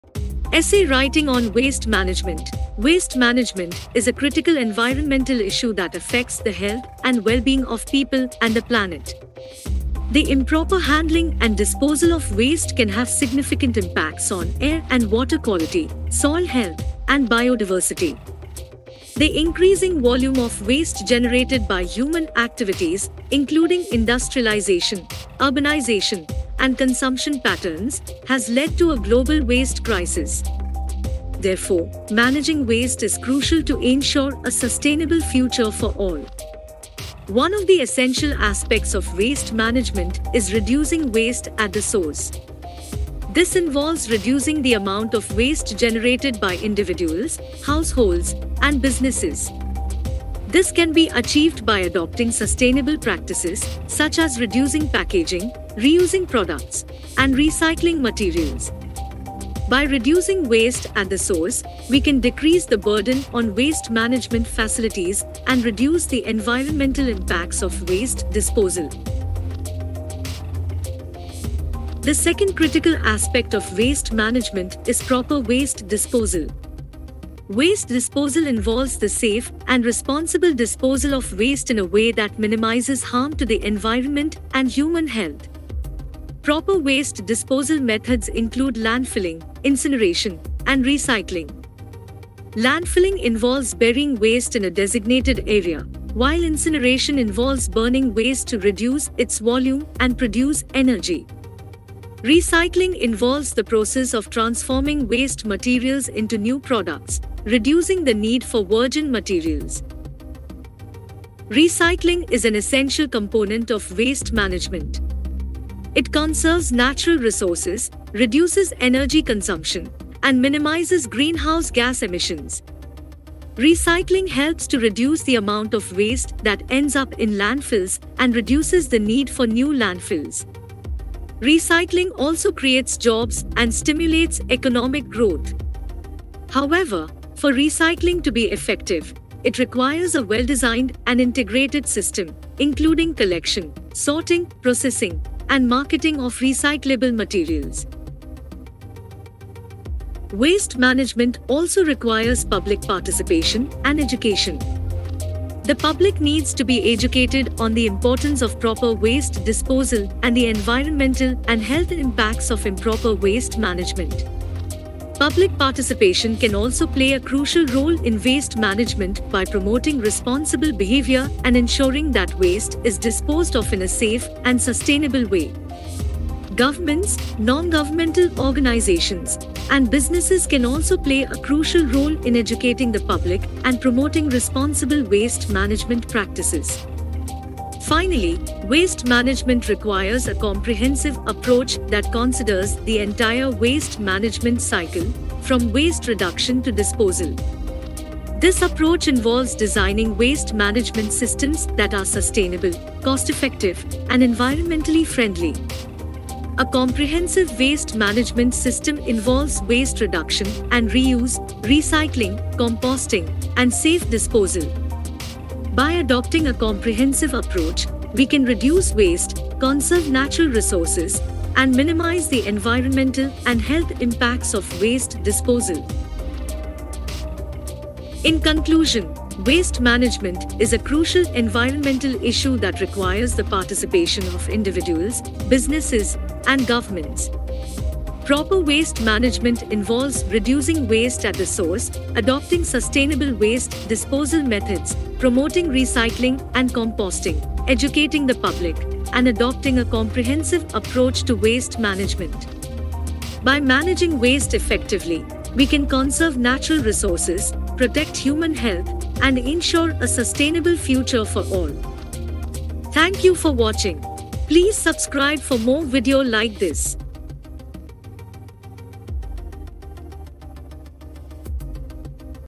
Essay Writing on Waste Management
In this video, we will guide you on how to write a well-structured essay on the topic of waste management. Waste management is an essential aspect of environmental sustainability, and writing an essay on this topic can help raise awareness and promote positive change.

Writing an essay on waste management may seem challenging, but with our expert tips and guidance, you will be able to effectively express your thoughts and ideas on this important topic. We will cover the basic structure of an essay and provide you with essential vocabulary and phrases to help you articulate your points clearly.

By the end of this video, you will have gained a deeper understanding of the importance of waste management, the various types of waste, and the different methods of waste disposal. You will also have the skills and confidence to write a well-crafted essay that can inspire others to take action and make a difference.

Thank you for choosing Education in Second as your go-to educational platform. Let's dive into the world of waste management and start writing!